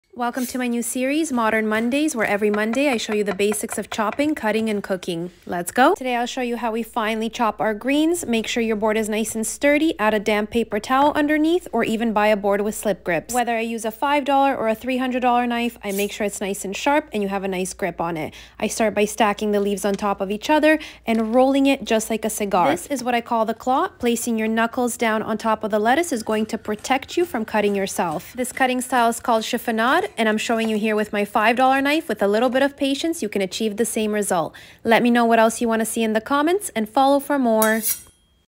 Episode 1 — HOW TO Cut lettuce super thin
Welcome to my new series “ Modern Mondays ” Episode 1 — Today I’m showing you how to thinly slice lettuce, basil, spinach and many other leafy greens with this technique. These types of videos have been the most requested on my page and I’m constantly being asked to show you how I cut lettuce, onions, how to poach eggs, and give you overall tips and advice in the kitchen. Now, you all know that I am self-taught and never went to culinary school but I feel like over the last decade + I have learned a lot through practice so I hope I am able to give you some valuable tips and tricks. Truth is, my mom has always cut lettuce like this and her and I have a very hard time eating salads that have a thicker cut. I personally feel like when you take the time to just relax and have some patience and enjoy the process, the salad tastes that much better when it’s cut like this. This tutorial goes out to my Mom as she taught me how to cut like this. Whether you use a $5 knife which is what we’ve always used, or a $300 knife, the principles remain the same. A more expensive knife does make life much easier but not needed for a delicious outcome.

Instructions:
1) Make sure the board you are using is not moving around. Either purchase a board with grips, use a non-slip mat or use a damp paper towel underneath.
2) A sharp knife is a must here. A dull knife will bruise most produce and you are more likely to cut yourself. Wash the romaine lettuce leaves and pat them dry.
3) Stack 3-4 leaves on top of each-other and roll them up like a cigar and start making thin cuts with a sharp knife. This technique is called Chiffonade and it is a slicing technique in which leafy green vegetables such as spinach, or flat-leaved herb like basil, are cut into long, thin strips.
4) This cut is accomplished by stacking leaves, rolling them tightly, then slicing the leaves perpendicular to the roll.